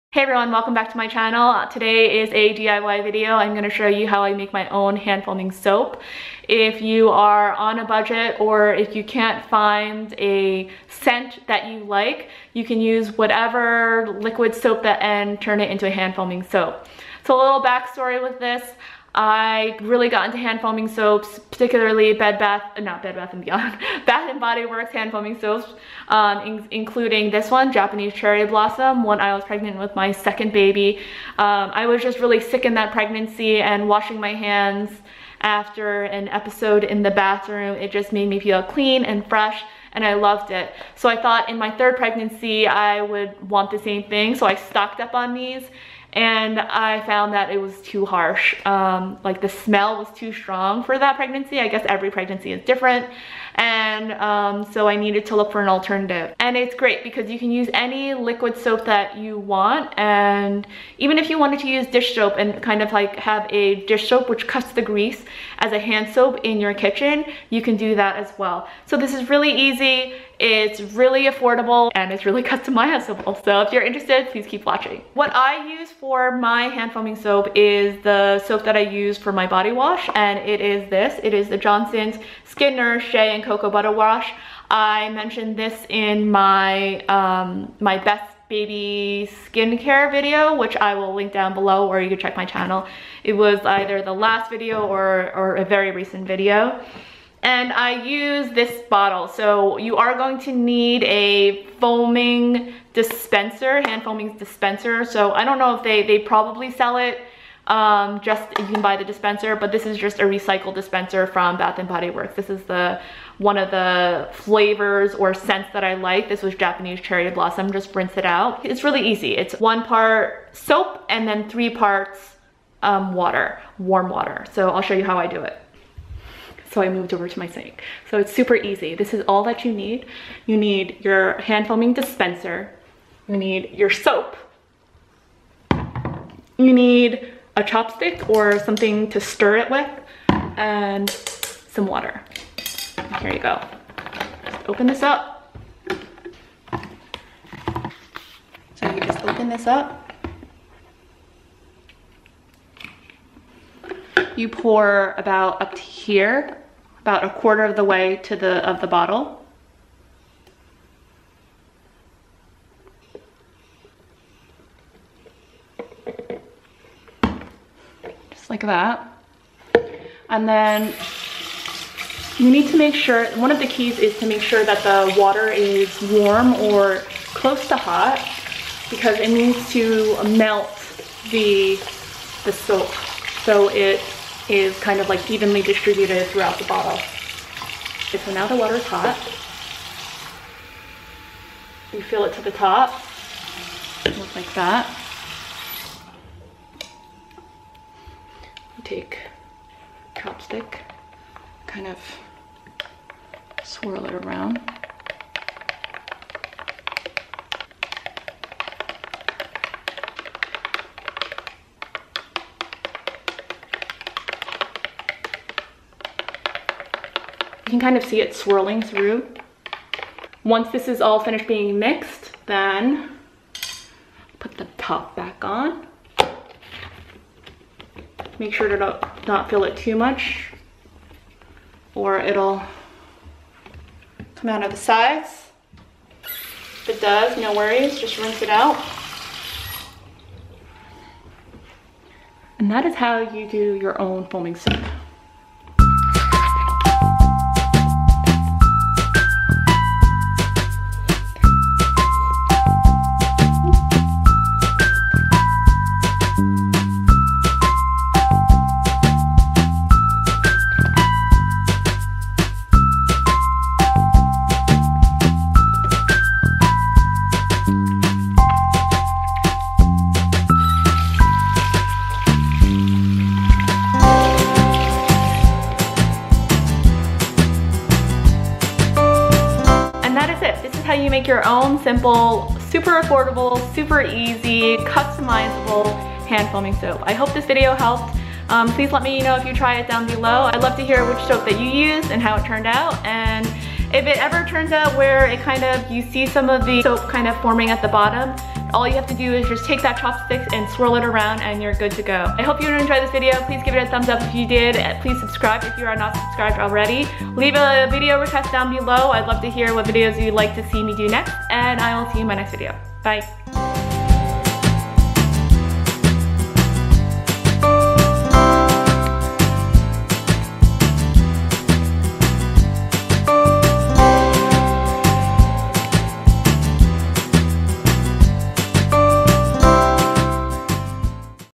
How To Make Hand Foaming Soap! DIY
Love making my own hand foaming soap. You can use this how-to 'recipe' and turn any liquid soap into a foaming one. Let me know if you try this! xo

Mentioned this in my baby skincare video (link below).

Share, Comment, and Thumb this video too! :)

H E A L T H Y B U T J U I C Y!